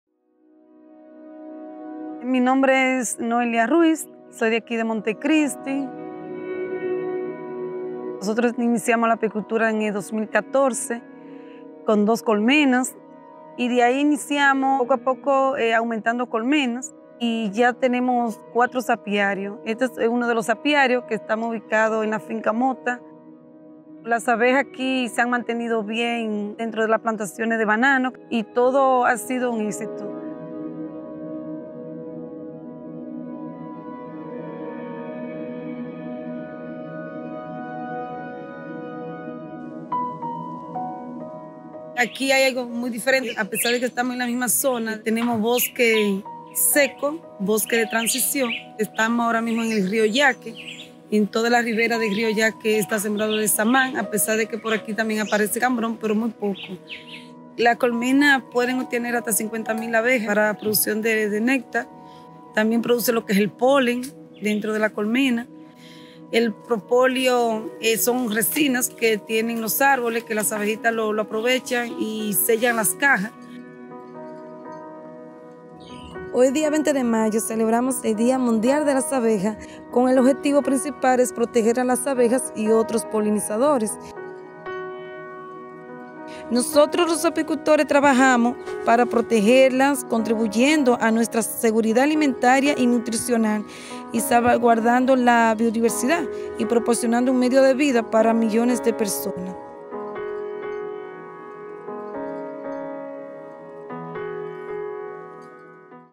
¡Tenemos un compromiso con las abejas!
Entre las funciones que hacen las abejas y los distintos polinizadores están:

🍌Aumentar la producción de alimentos
🌸Mantener la biodiversidad
🌎Prevenir riesgos ambientales

En República Dominicana contamos con hombres y mujeres que se dedican a la apicultura, trabajando día tras día contribuyendo a la seguridad alimentaria del país.

👩🏻‍🌾Noelia Ruíz
📍 Guayubín, Monte Cristi